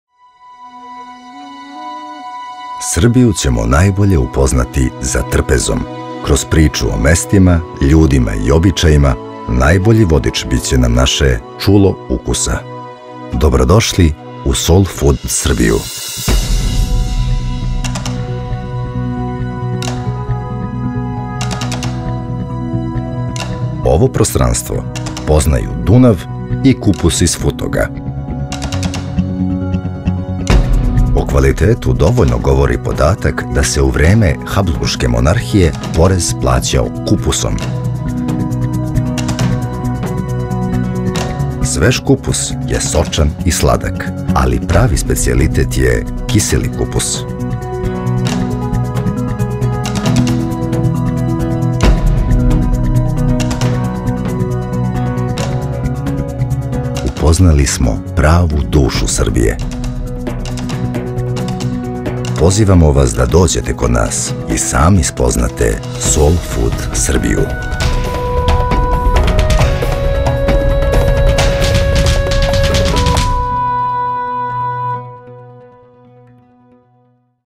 Soulfood Srbija - Futoški kupus
Vojvodina se nalazi na severu Srbije. To je ravničarska oblast usamljenih salaša, bogatih sela i gradova srednjoevropske arhitekture i kulinarske tradicije. Jedan od glavnih ratarskih proizvoda koji istorija pamti od XVI veka jeste zdrav i ukusan futoški kupus koji je ime dobio po Futogu pored Novog Sada. Kupus se u Vojvodini priprema kao glavno jelo ili dodatak, kao
fil za slane pite ili palačinke ili kao čorba, a čuveni kiseli kupus, osim što se jede kao salata, osnova je čuvene sarme i još čuvenijeg podvarka. Sarma, to su zamo tuljci začinjenog mlevenog mesa i pirinča u listovima kiselog kupusa, a podvarak
je dinstani, sitno iseckani kiseli kupus koji se najčešće služi uz kolenicu ili ćurku, gusku, plovku, a neretko i ribu.